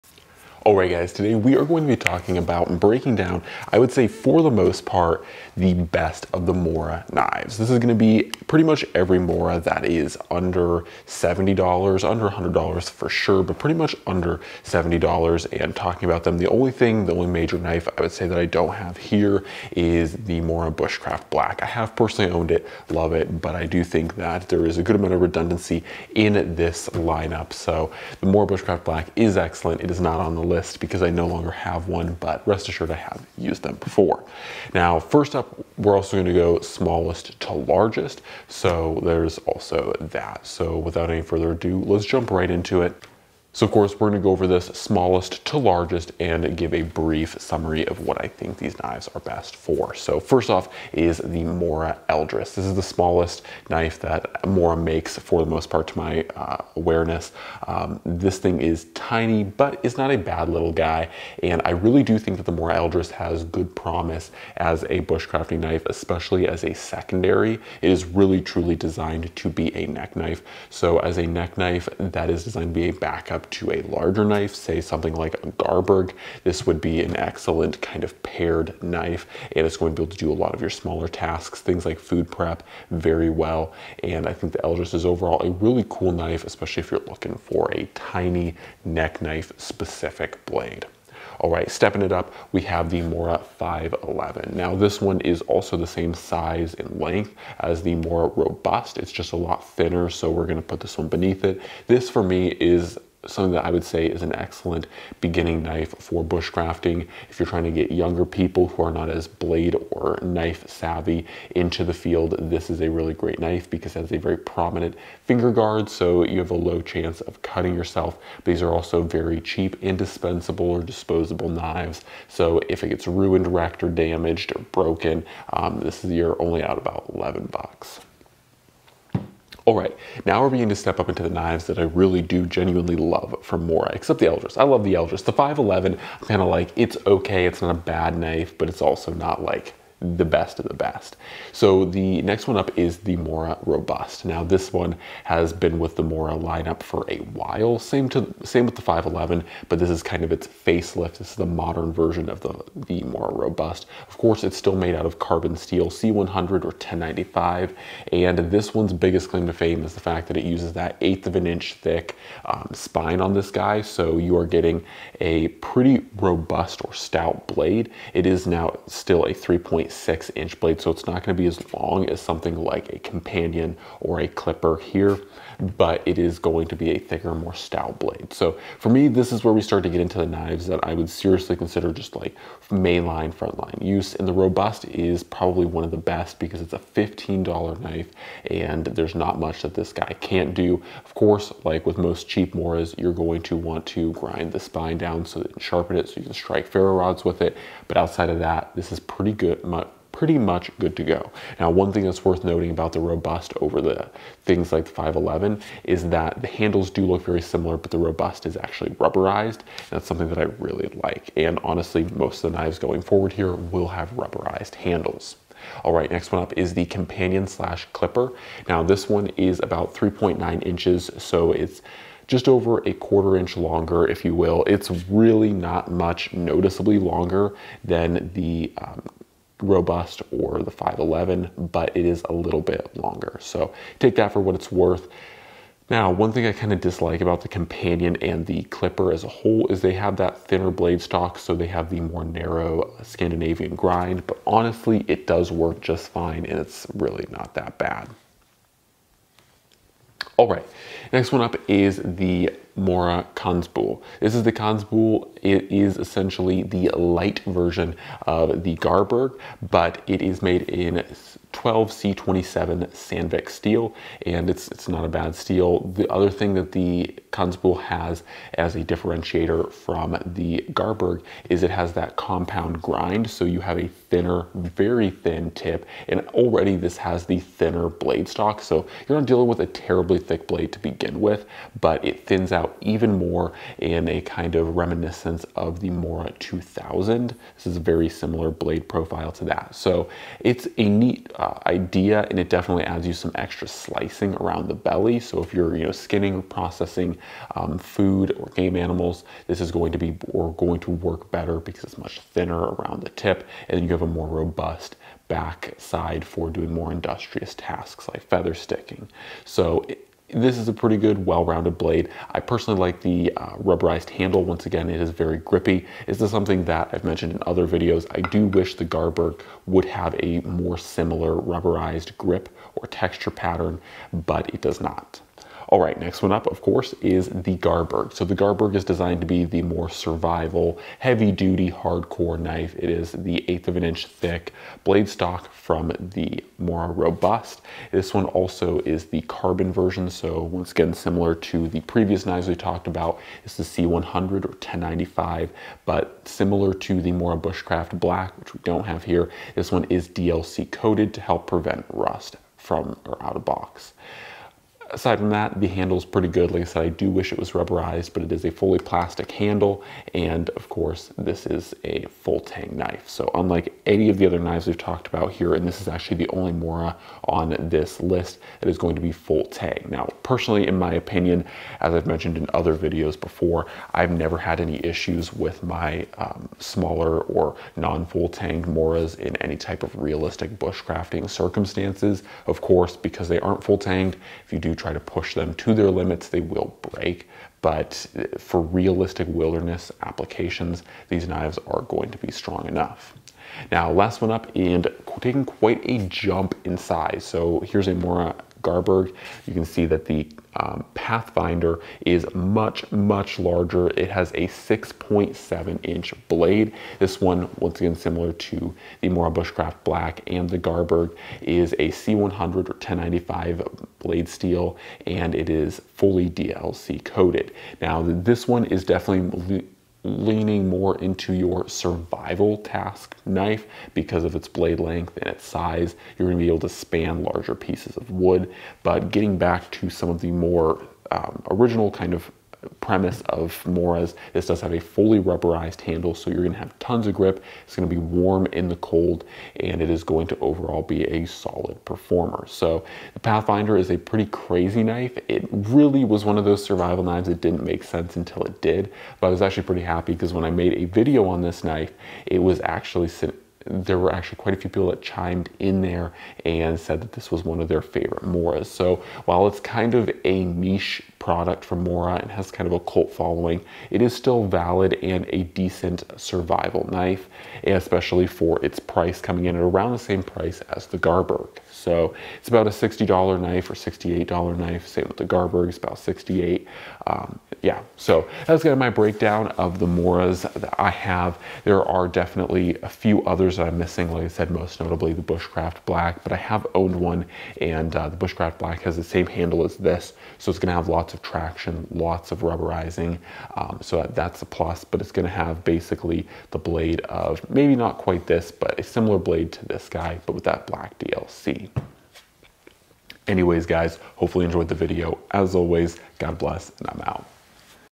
The Best Morakniv Knife Breakdown on YouTube
This video is quite comprehensive going over all the most important Morakniv knives for bushcraft.